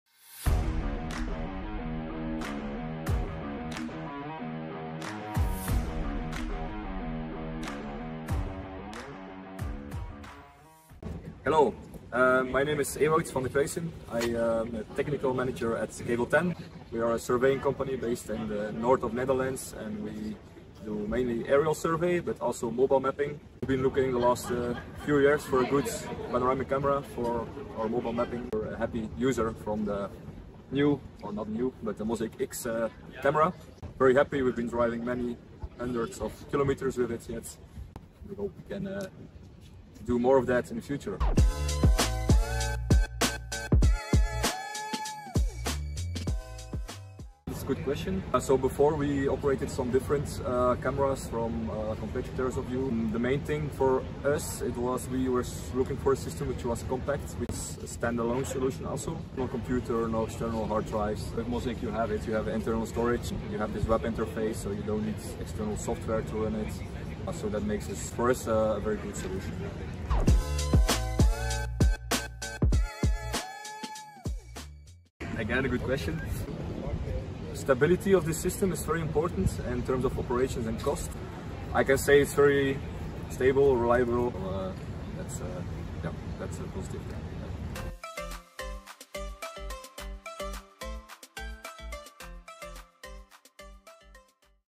Kavel 10's Review: Mosaic X 360° Camera Transforms Mapping, Surveying and Data Collection
Ewoud Van Der Cruyssen from Kavel 10 shares his insights on the exceptional advantages of Mosaic Camera Systems.

Dive into how the Mosaic X mobilemapping camera has transformed their surveying operations, enhancing precision in infrastructure projects and streamlining data integration.

Experience the efficiency and user-friendliness of the Mosaic X Camera System, a benchmark in data collection and operational stability. Curious about the technical specifics?

A big thank you to Ewoud for his candid review!